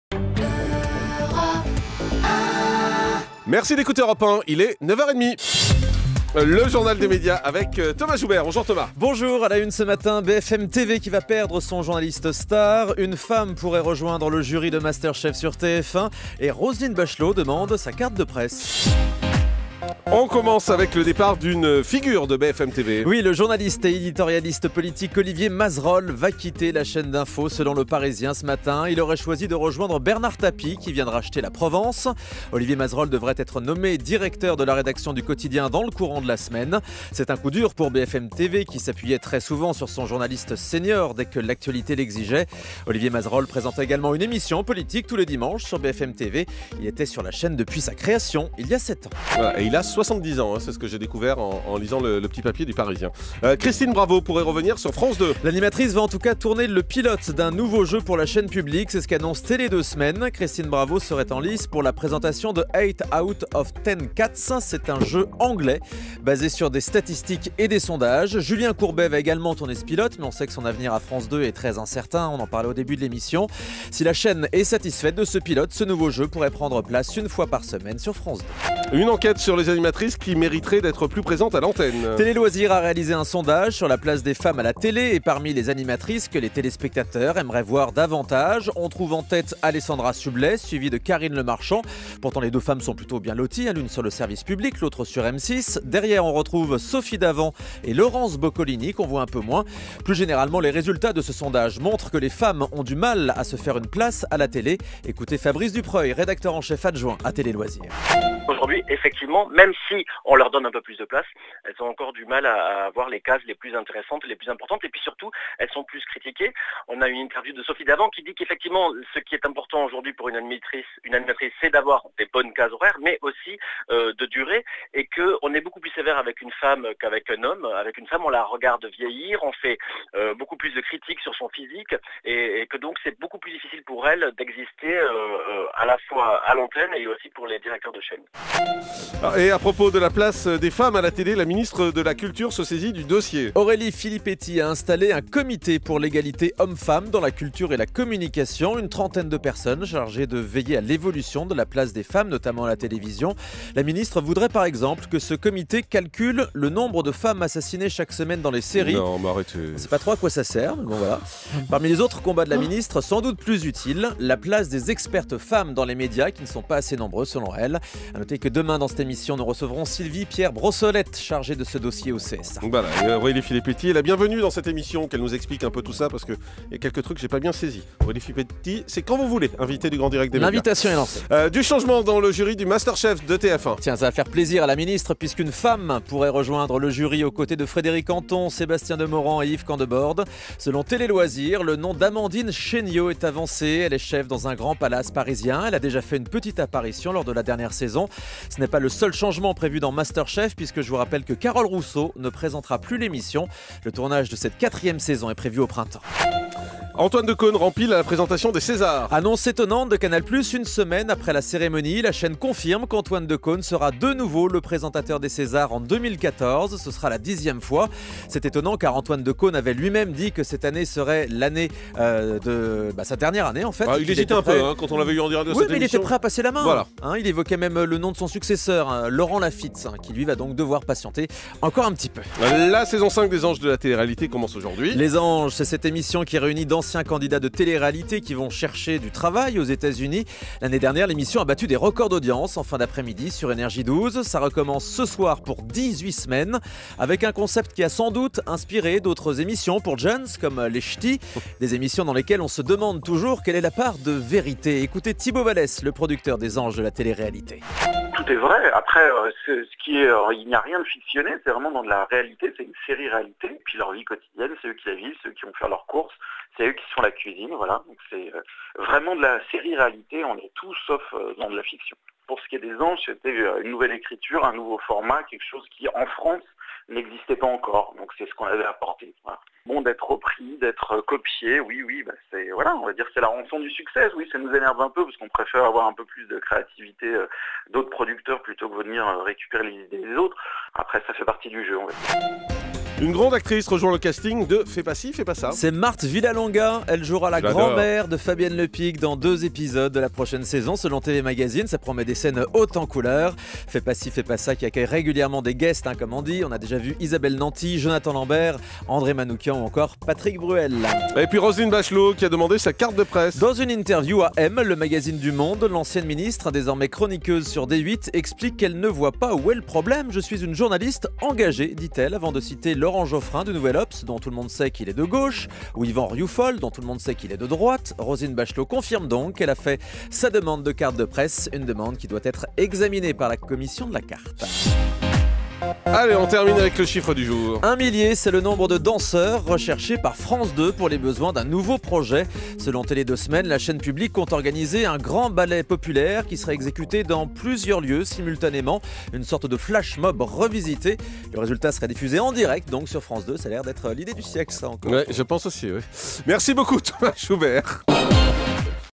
Une femme dans le jury de Masterchef ?
Selon Télé Loisirs, la chef du Palace Amandine Chaignot pourrait rejoindre le jury de MasterChef.